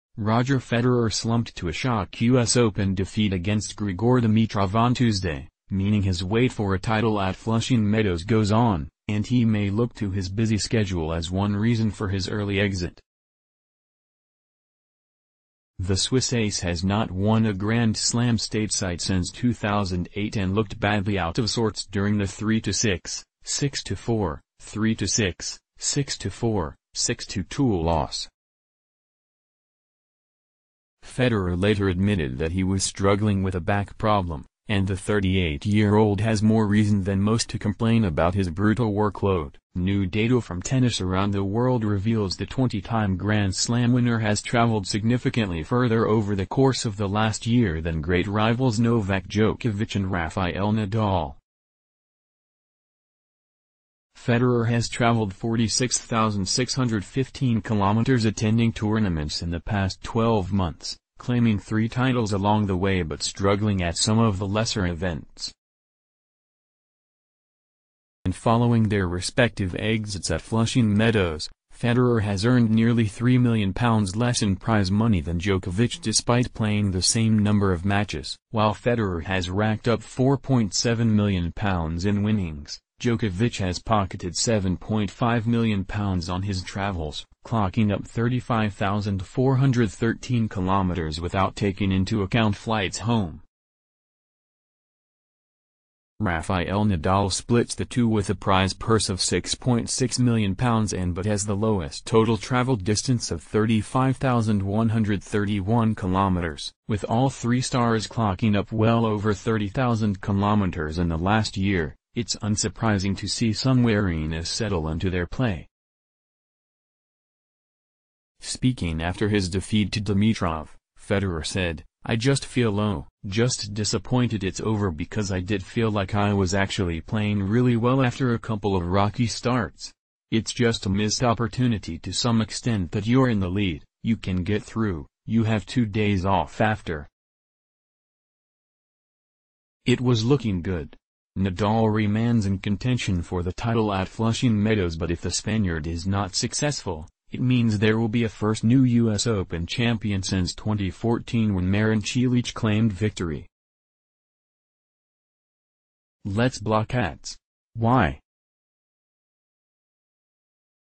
Rafael Nadal and Novak Djokovic held one key advantage over Roger Federer ahead of US Open | News...
Roger Federer slumped to a shock US Open defeat against Grigor Dimitrov on Tuesday, meaning his wait for a title at Flushing Meadows goes on – and he may look to his busy schedule as one reason for his early exit. The Swiss ace has not won a Grand Slam Stateside since 2008 and looked badly out-of-sorts during the 3-6, 6-4, 3-6, 6-4, 6-2 loss. Federer later admitted that he was struggling with a ba...